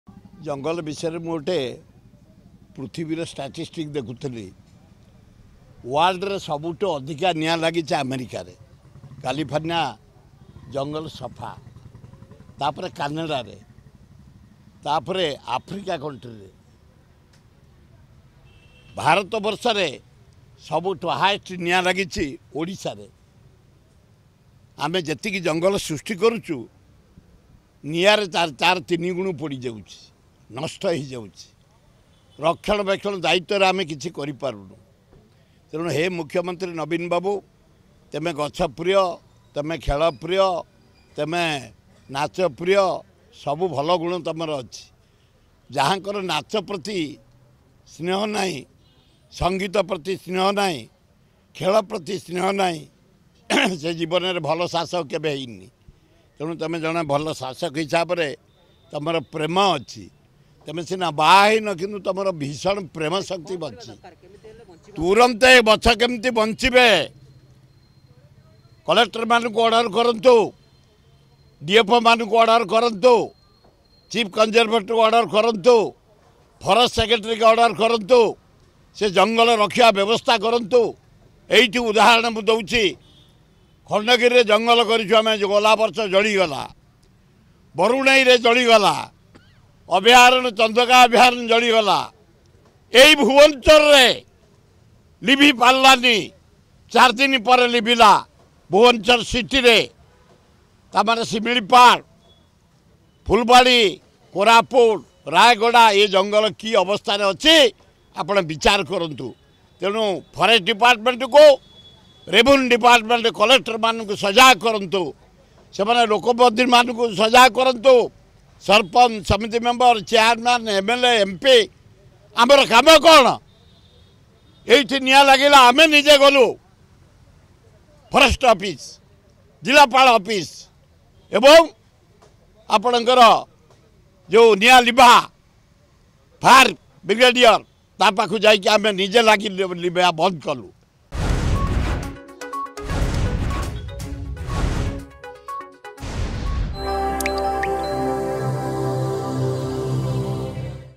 ହେ ନବୀନ ବାବୁ, ତୁମର ଭୀଷଣ ପ୍ରେମ ଶକ୍ତି ଅଛି : ସୁର ରାଉତରାୟ
ହେ ନବୀନ ବାବୁ, ତୁମେ ସିନା ବାହା ହୋଇନ କିନ୍ତୁ ତୁମର ଭୀଷଣ ପ୍ରେମ ଶକ୍ତି ଅଛି : ସୁର ରାଉତରାୟ